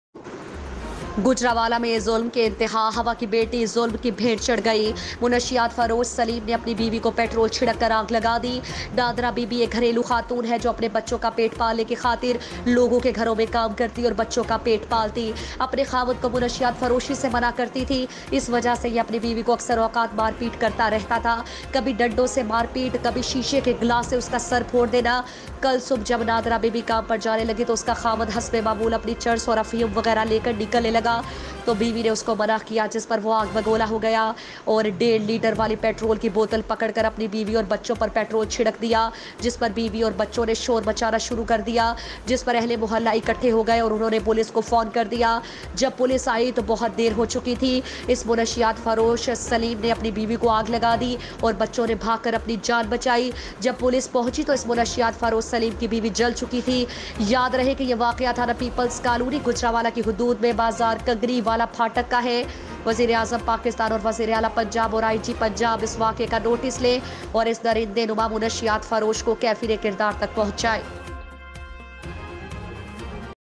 ظلم کی انتہا حوا کی بیٹی ظلم کی بھینٹ چڑھ گئیت فروش سلیم نے اپنی بیوی کو پٹرول چھڑک کر آگ لگا دی
گوجرانوالہ میں ظلم کی انتہا حوا کی بیٹی ظلم کی بھینٹ چڑھ گئی
منشیات فروش سلیم  نے اپنی  بیوی کو پٹرول چھڑک کر آگ لگا دی

 نادرہ بی بی ایک گھریلو خاتون ھے جو اپنے بچوں کا پیٹ پالنے کی خاطر لوگوں کے گھروں میں کام کرتی اور بچوں کا پیٹ پالتی

اپنے خاوند کو منشیات فروشی سے منع کرتی تھی اس وجہ سے یہ اپنی بیوی کو اکثر اوقات مار پیٹ کرتا رھتا تھا کبھی ڈنڈوں سے مار پیٹا کبھی شیشے کے گلاس سے اس کا  سر پھوڑ دینا کل صبع  جب نادرہ بی بی کام پر جانے لگی تو اس کا خاوند حسب معمول اپنی چرس اور افیم وغیرہ لے کر نکلنے لگا تو  بیوی نے اس کو منع کیا جس پر وہ آگ بگولہ ھو گیا اور ڈیڑھ لیٹر والی پیٹرول کی بوتل پکڑ کر اپنی بیوی اور بچوں پر پٹرول چھڑک دیا جس پر بیوی اور بچوں نے شور مچانہ شروع کر دیا جس پر اھل محلہ اکٹھے ھو گئے اور انھوں نے پولیس کو فون کر دیا جب پولیسں آئی تو بہت دیر ھوچکی تھی اس منشیات فروشی سلیم نے اپنی بیوی کو آگ لگا دی اور بچوں نے بھاگ کر اپنی جان بچائی جب پولیسں پھنچی تو اس منشیات فروش سلیم کی بیوی جل چکی تھی یاد رھے یہ واقعہ تھانہ پیپلز کالونی گوجرانوالہ کی حدود میں بازار کنگنی والا پھاٹک کا ھے وزیر اعظم پاکستان اور وزیر اعلی پنجاب اور آئی جی پنجاب اس واقعہ کا نوٹس لیں اور اس درندے نما منشیات فروش کو کیفیر کردار تک پہنچائیں